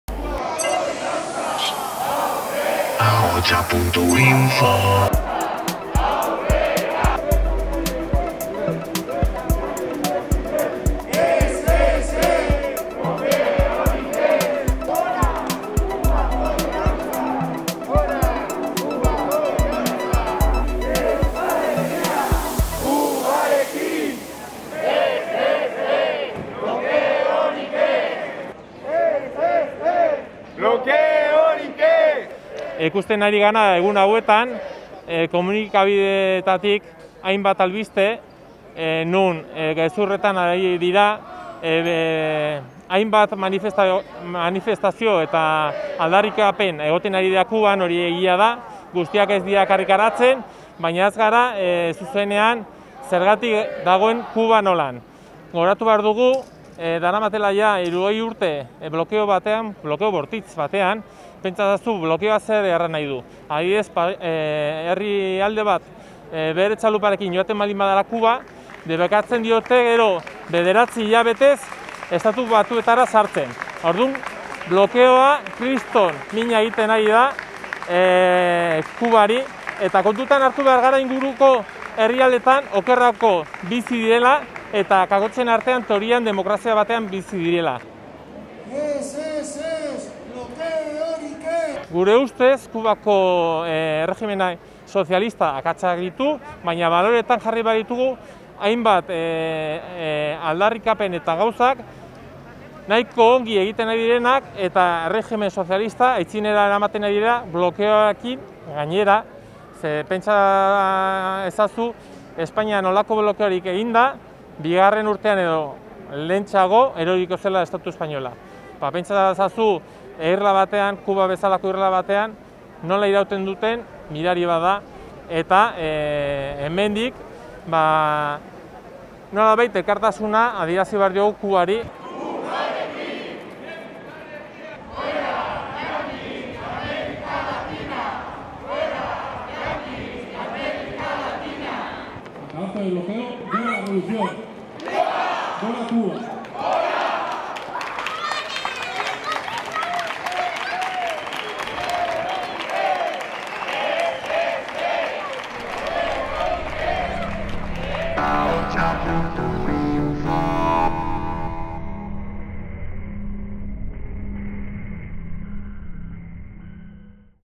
Kubari AEBk ezarritako blokeoa bertan behera uztea eskatu dute Iruñean
Azken egunetan Kuban izandako protesta eta liskarrak direla eta, Karibeko irlari ezartzen zaion blokeoa hizpide izan dute gaur Iruñean bildutako lagunek. Izan ere, Kubaren egoera sozioekonomikoa blokeo honen emaitza dela salatu dute, eta zigor honekin amaitzeko eskatu dute.